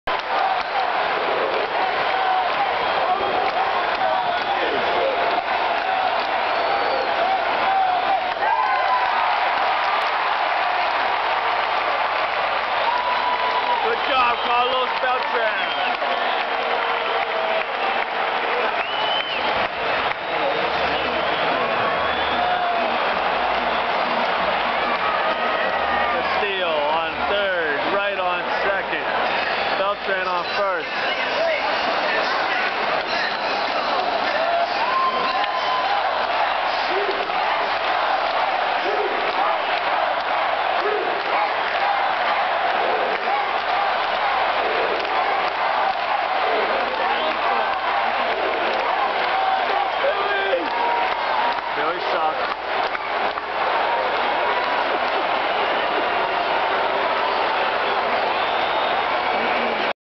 Carlos Beltran Goes For A Walk
Carlos Beltran gets walked to load the bases during the New york Mets vs the San Dieago Padres.  8-23-07